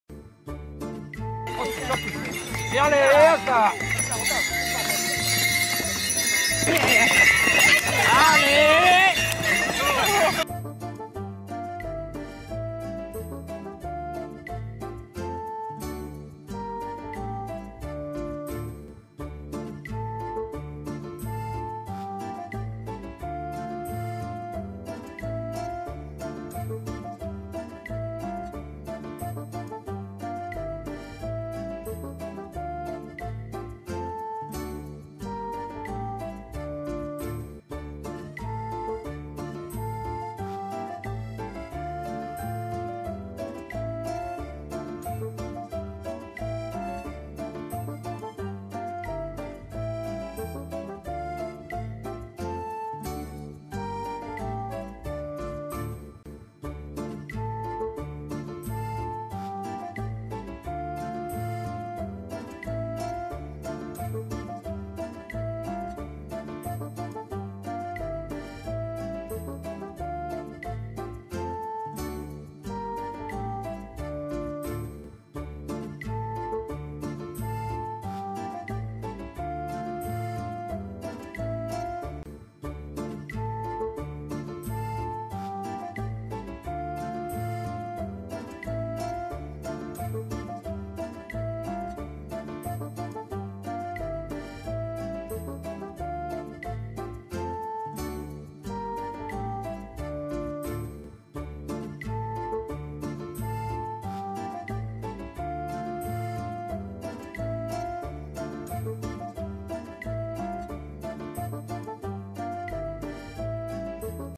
Grudziądzki kulig na tle zabytkowych Spichrzów
W Grudziądzu wyjątkowy kulig na tle zabytkowych Spichrzów nad Wisłą. Przejażdżka i oczywiście gorąca grochówka i herbata na rozgrzewkę.